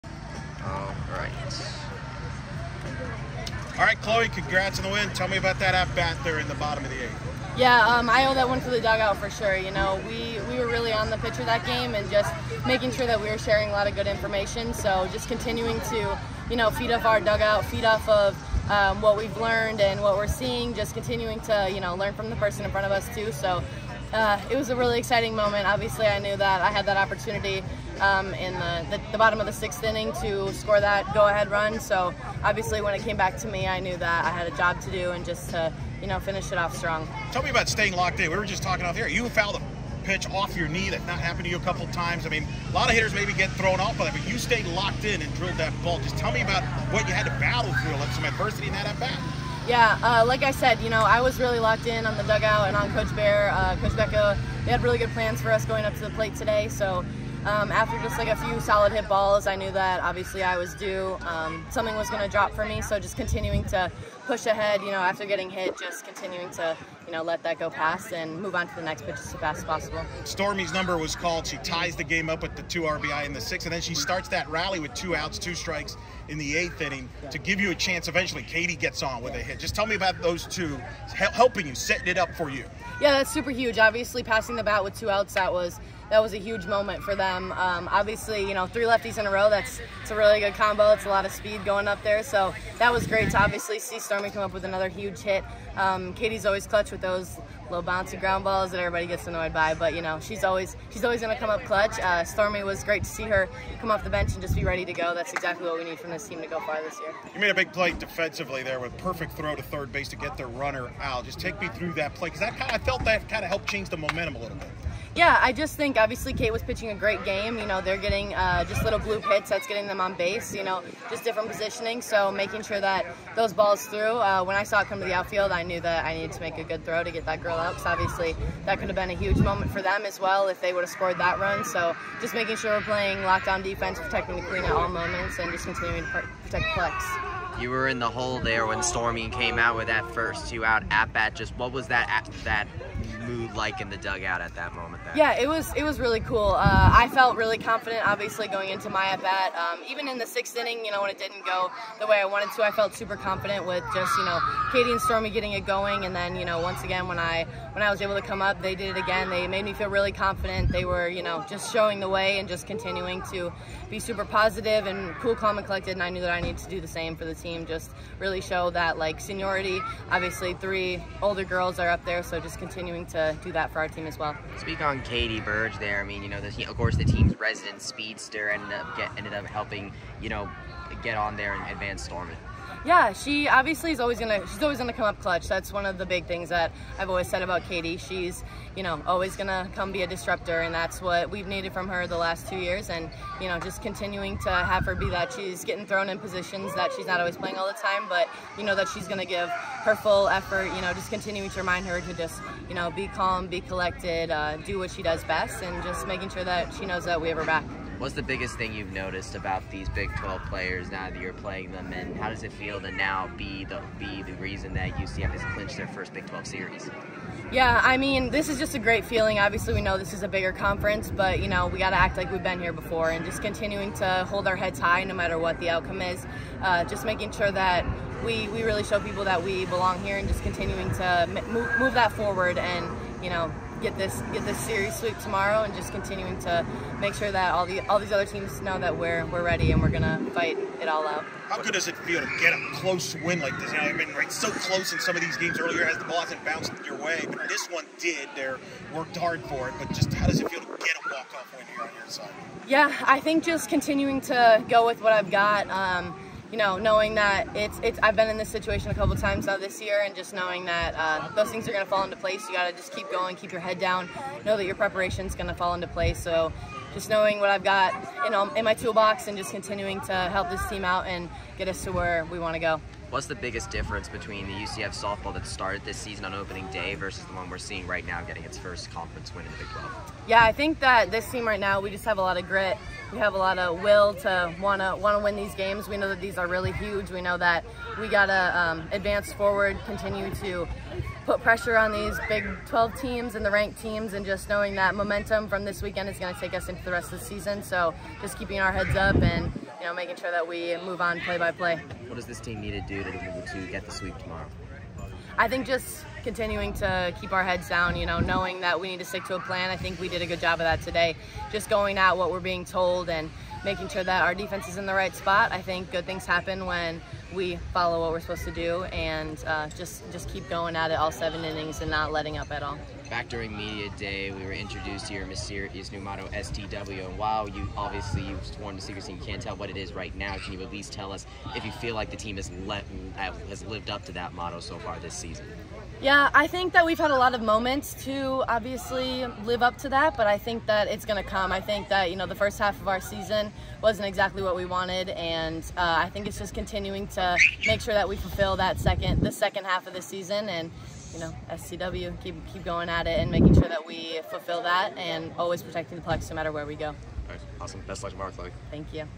UCF Softball Postgame Availability vs. Kansas - Chloe Evans
Right fielder Chloe Evans discusses the Knights' 3-2 walk-off win over Kansas March 9, 2024.